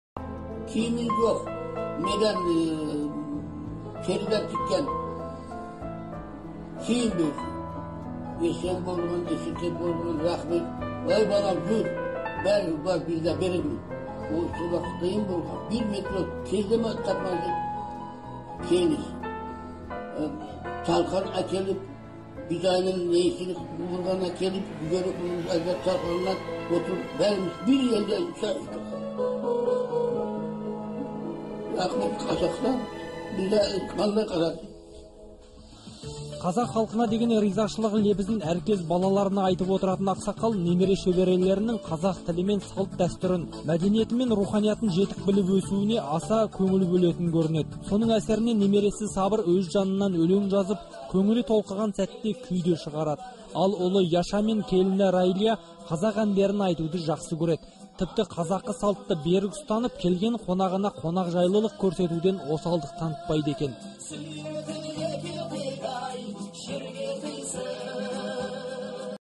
AHISKAproduction Дедушка выражает благодарность казахскому народу
Дедушка Гусейнов Иса Назимович, выражает благодарность казахскому народу .